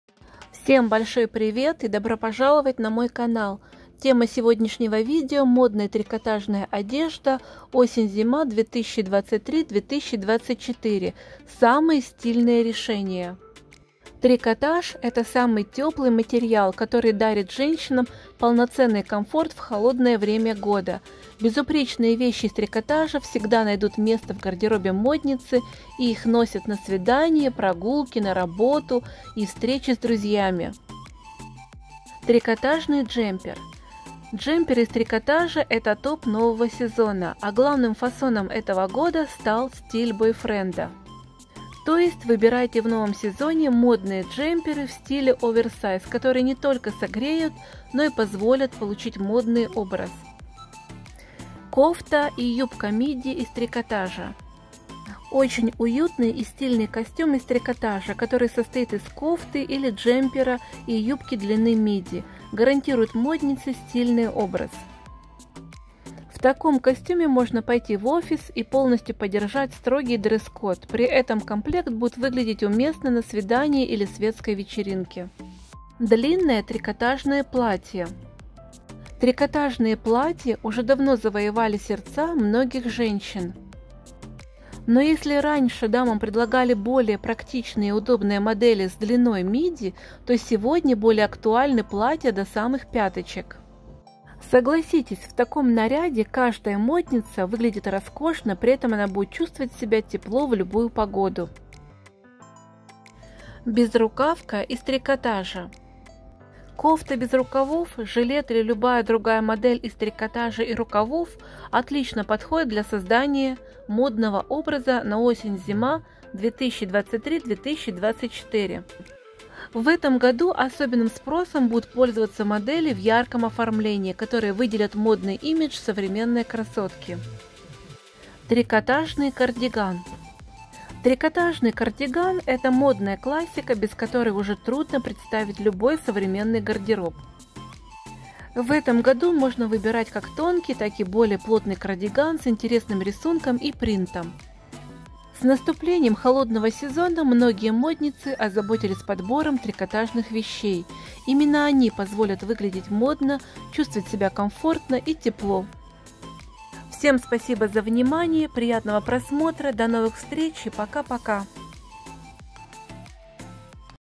МОДНАЯ ТРИКОТАЖНАЯ ОДЕЖДА ОСЕНЬ-ЗИМА 2023-2024 САМЫЕ СТИЛЬНЫЕ РЕШЕНИЯ
Трикотаж – это самый теплый материал, который дарит женщинам полноценный комфорт в холодное время года. Безупречные вещи из трикотажа всегда найдут место в гардеробе модницы, их носят на свидания, прогулки, на работу и встречи с друзьями. Сегодня мы покажем, какие трикотажные вещи будут популярны в сезоне осень-зима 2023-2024.